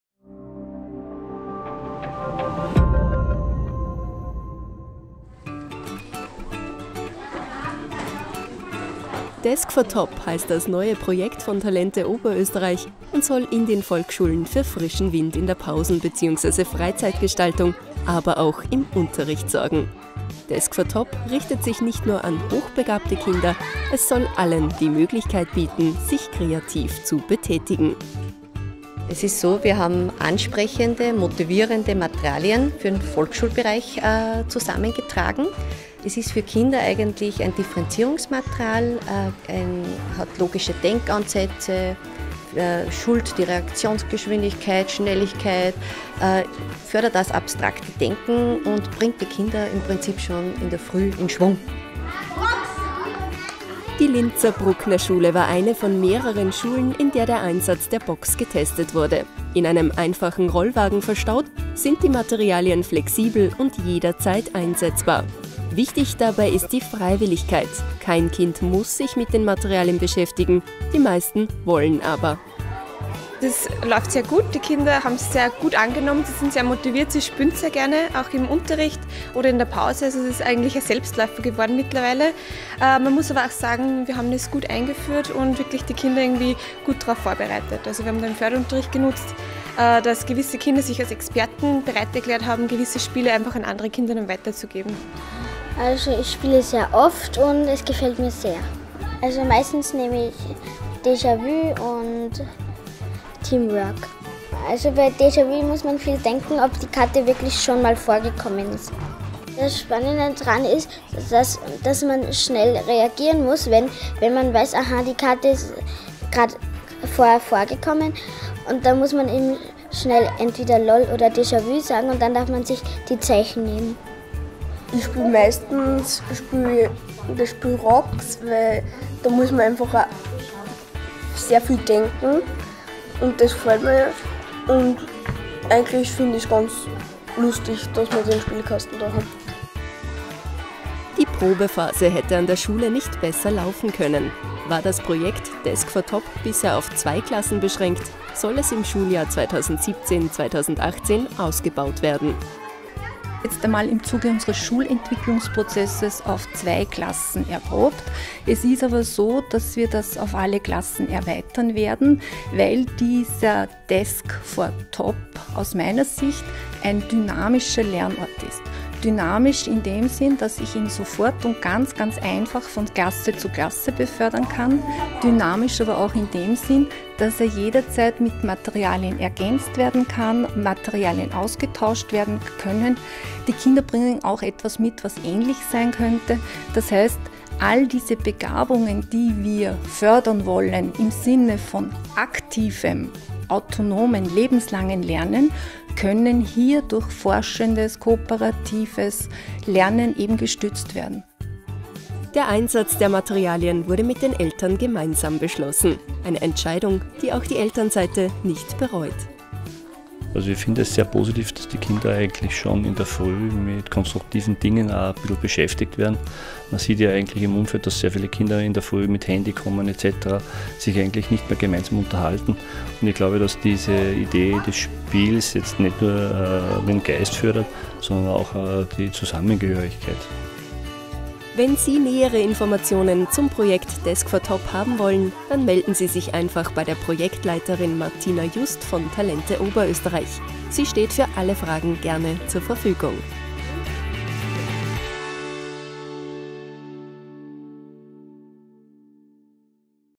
Desk for Top: Die Pausen sinnvoll nutzen
Desk for Top ist das neue Projekt von Talente Oberösterreich, in dem Volksschulkindern herausfordernde und ansprechende Spiele und Materialien in der Frühbetreuung, Pausen oder auch im Unterricht zur Verfügung gestellt werden. Ziel ist es, auf freiwilliger Basis, systemische Denkansätze und logisches Schlussfolgern zu fördern sowie Reaktion und Schnelligkeit zu schulen. Die Spiele sind für alle Kinder verwendbar, egal ob hochbegabt oder nicht. In einem Rollwagen aufbewahrt, lassen sich die Materialien problemlos transportieren. Wir waren in der Linzer Brucknerschule und haben uns angesehen, wie das Projekt "Desk for Top" bei Schülerinnen und Schülern, Lehrkräften und Eltern ankommt.